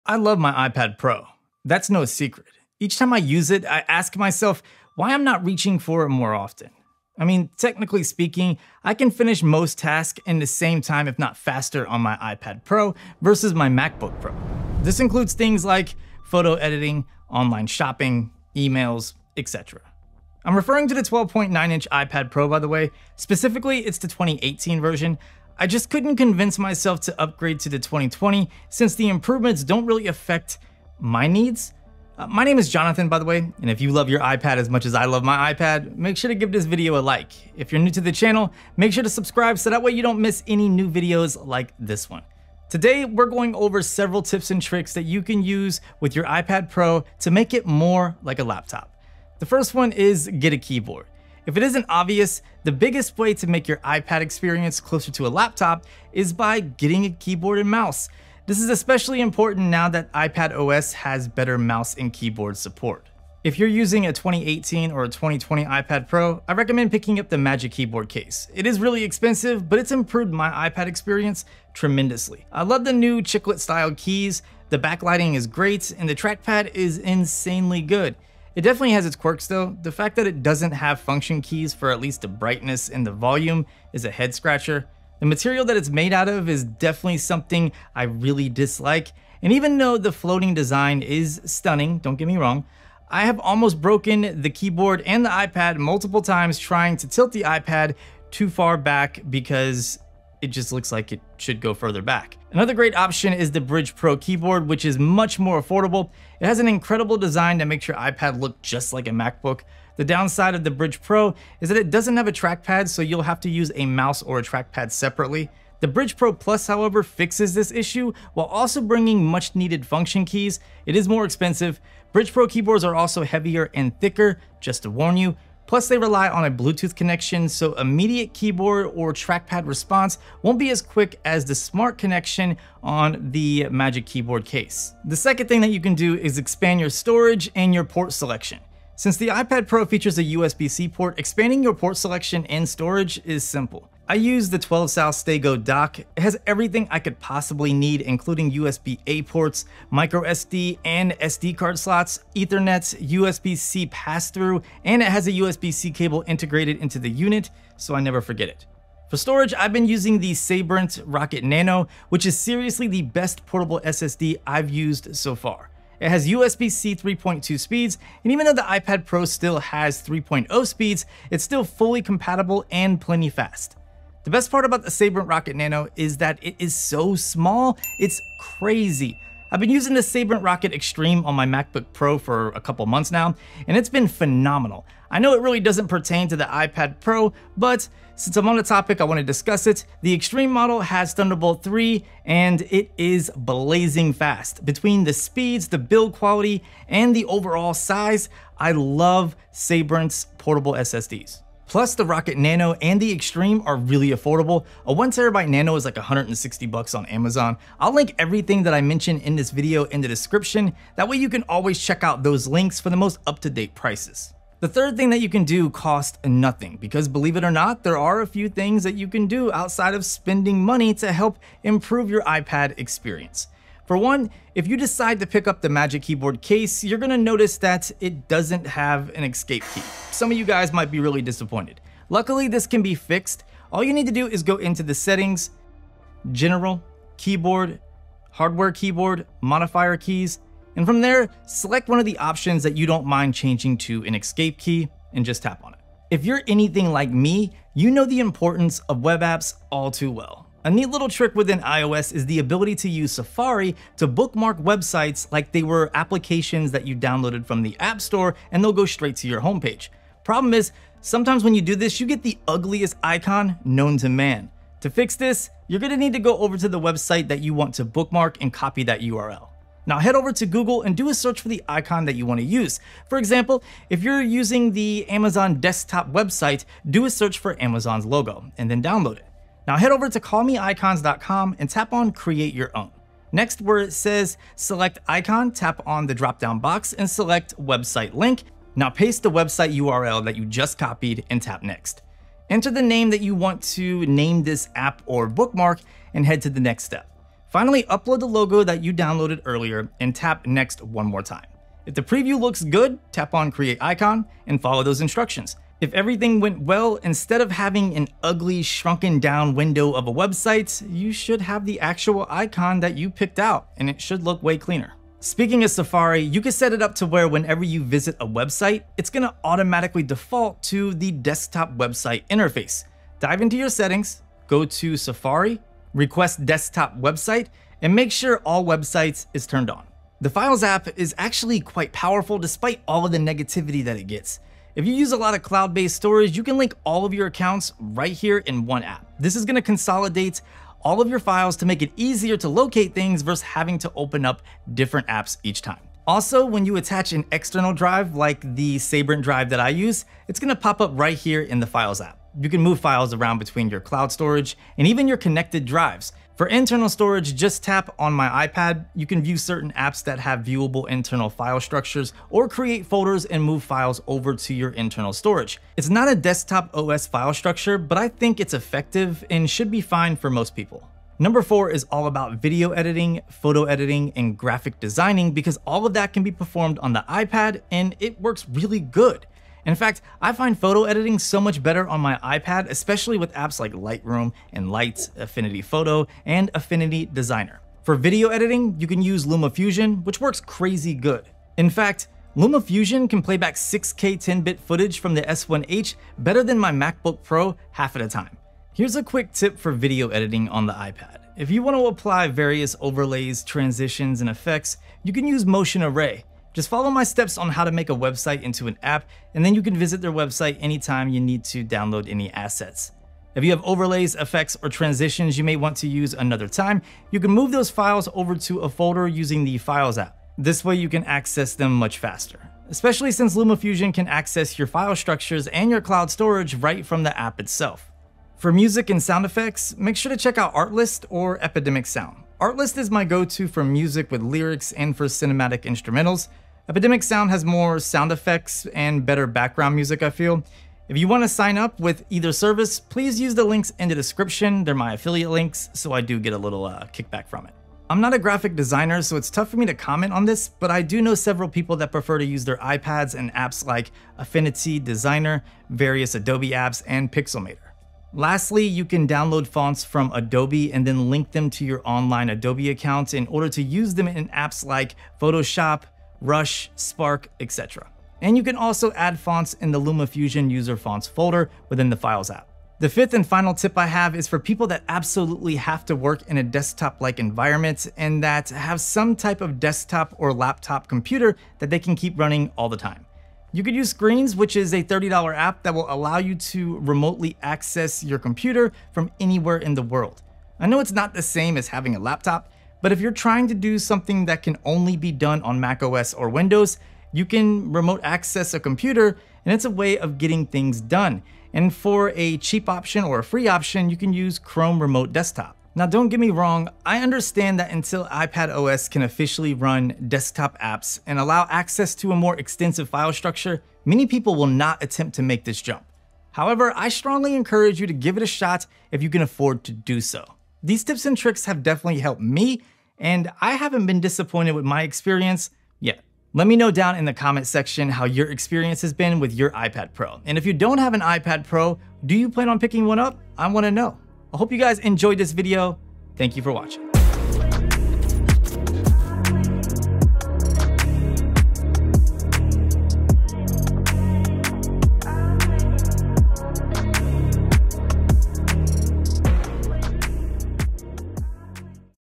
Tips To Turn Your iPad Pro Into A Laptop Replacement
In this video I'm going over tips to turn your iPad Pro into a laptop replacement. I'll be covering a few of my favorite accessories, favorite apps, and some simple tips and tricks that you can use to improve your experience. The information in this video applies to the iPad Pro (2018 and 2020). If you would like to see an iPad Pro 2020 unboxing, review, and tips and. tricks let me know and I will pick one up. Also, make sure to check out my iPad Pro vs Surface Pro 7 comparison.

iPad Pro Magic Keyboard Case
Brydge Pro
Brydge Pro+
Sabrent Rocket Nano
Sabrent Rock XTRM
Twelve South StayGo